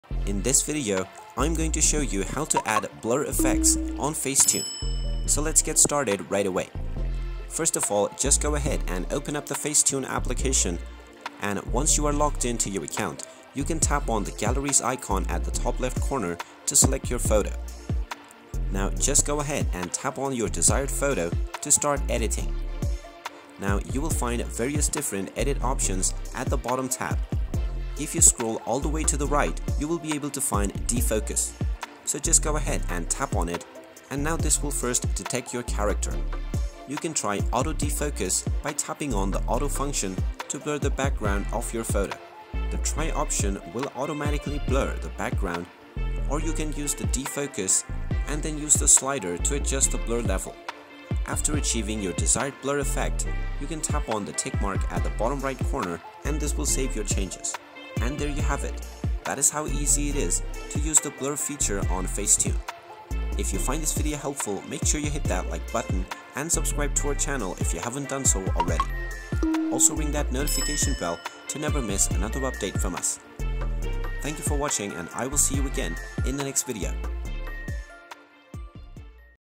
How to Add Blur Effects on Facetune 2023?
Elevate your photo editing skills with Facetune! Discover the art of adding mesmerizing blur effects to your images in this comprehensive tutorial. Whether you want to create dreamy portraits or enhance the background of your photos, our step-by-step guide will walk you through the process of adding stunning blur effects using Facetune's powerful tools. Join us and learn how to achieve professional-looking results that will make your photos stand out on social media and beyond.